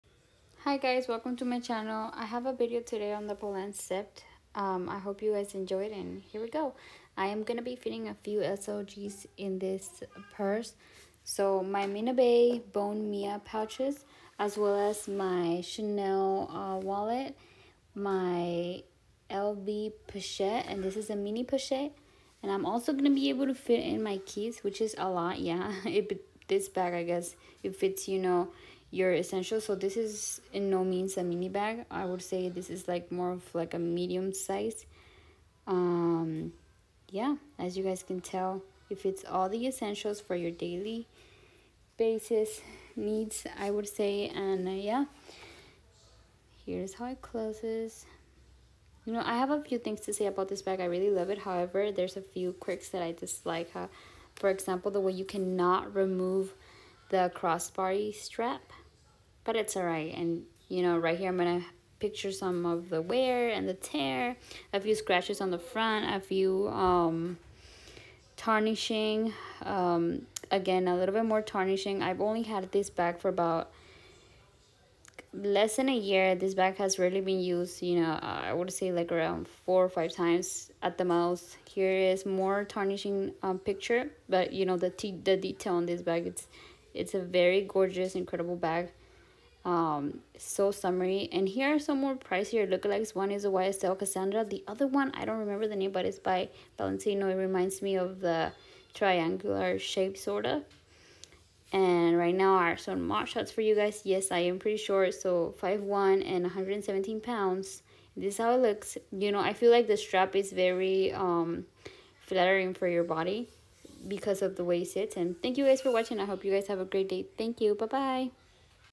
What fits in the polene sept!
In this video i’m showing you a bit of what fits in the polene sept and some wear and tear!
My pouches are from mina baie (women owned business!)
Mini pochette from LV
Chanel walket!